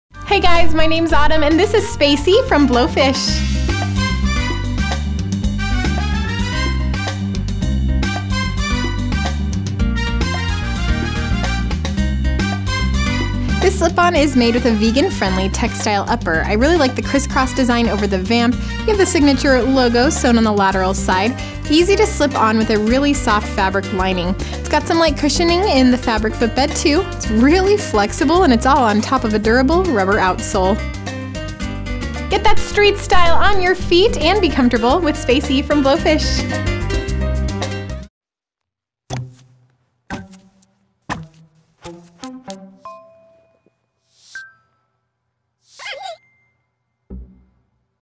Blowfish Spacey SKU:8844390
Take your look old school with the distressed look of the Blowfish® Spacey slip-on sneaker!
Vegan-friendly textile upper.
Easy slip-on wear.
Logo patch at heel panel.
Contrasting fabric lining.
Lightly cushioned fabric footbed.
Rubber outsole.
Imported.